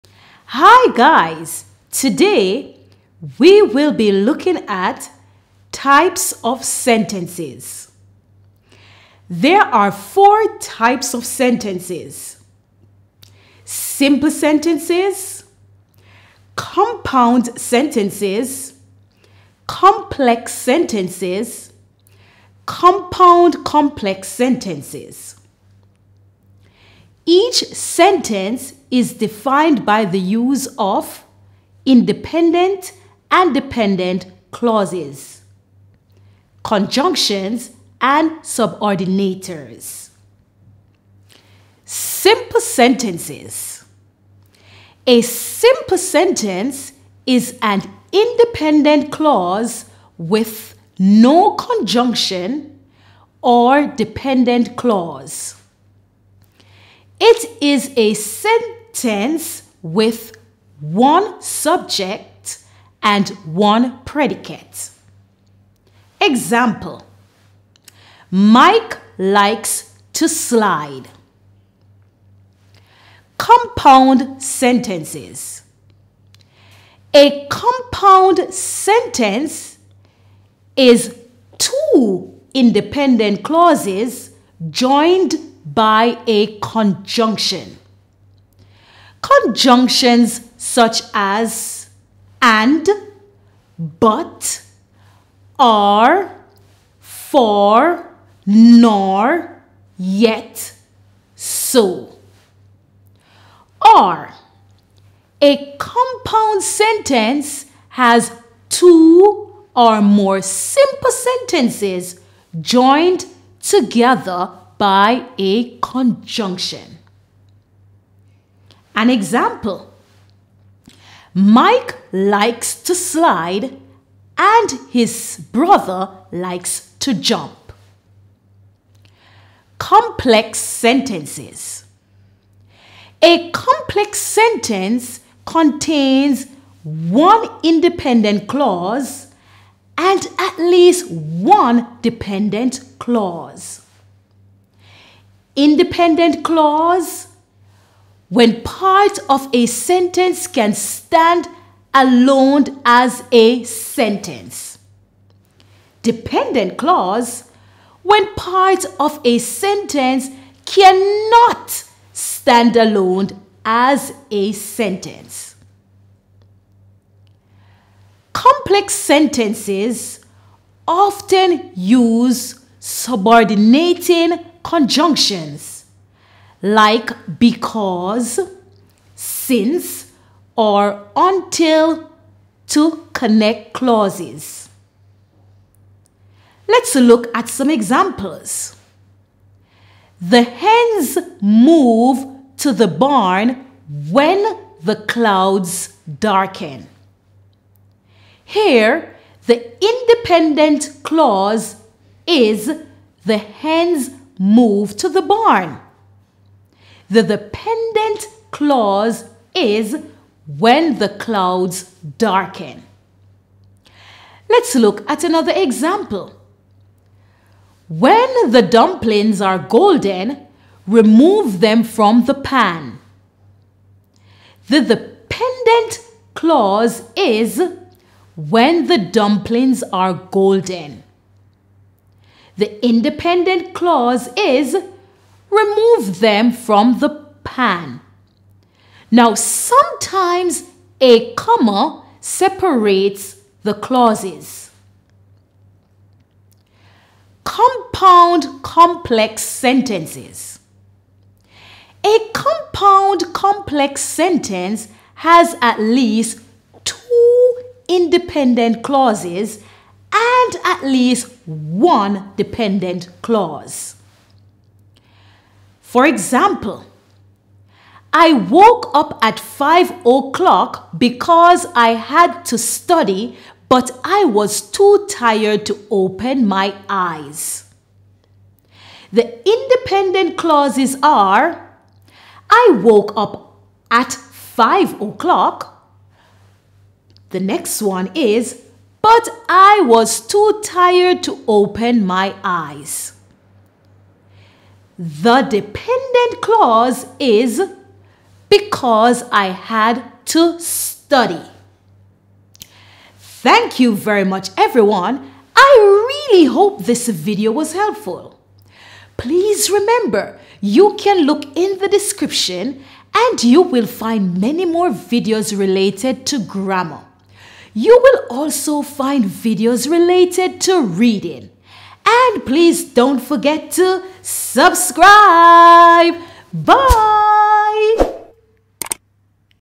4 Types of Sentences
There are four types of sentences:
Simple Sentences,
Compound Sentences,
Complex Sentences
, and Compound-complex Sentences.
Each sentence is defined by the use of independent and dependent clauses, conjunctions, and subordinators.